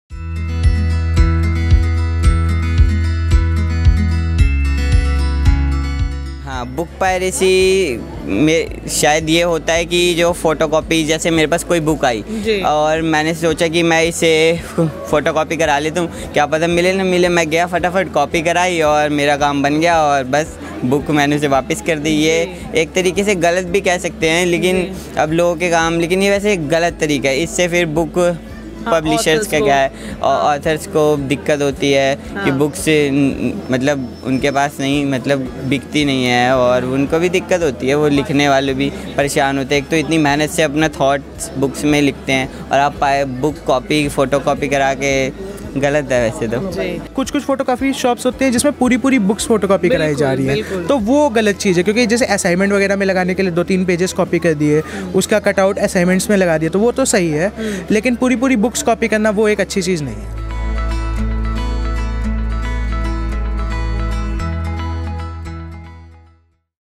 Students opinions on piracy?
Readers are the reason why every author gets motivated to write their new book. The response from readers is the heartbeat to an author. Especially, students play an important role in the bookish world.

They are the explorers of every genre! From Mythological to Sci-fic, they read and select out their favourites by exploring.
The pure essence of reading comes from reading the original books with real words. Unfortunately, nowadays book piracy is termite who is slowly eating the essence of the real books.

Imagine if books like Harry Potter or Game of thrones could get cancelled before reaching its conclusion part! Hard to imagine, right?
The impact of book piracy is serious and needs to be dealt with. Let's hear what these readers at worldbookfair thinks about the issue.